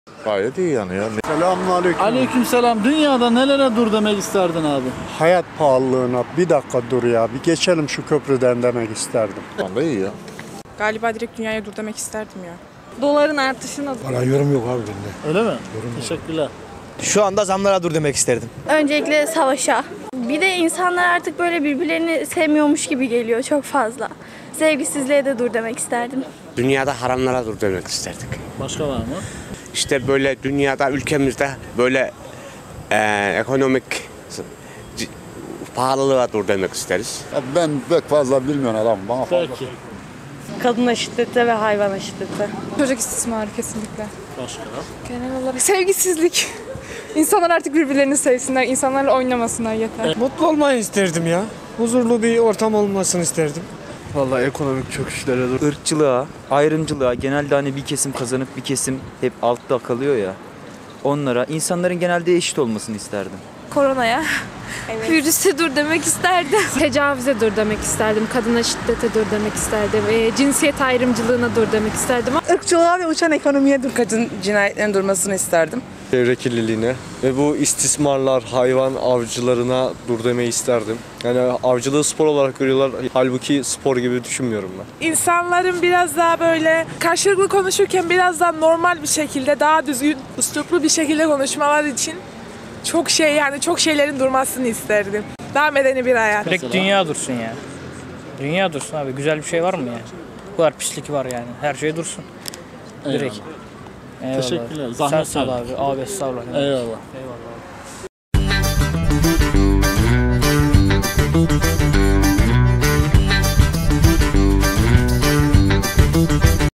Çankırı Artık Dur Diyor! - Sokak Röportajı
Dünya'da Nelere Dur Demek İstersiniz? Düşünenlere Selam Olsun:)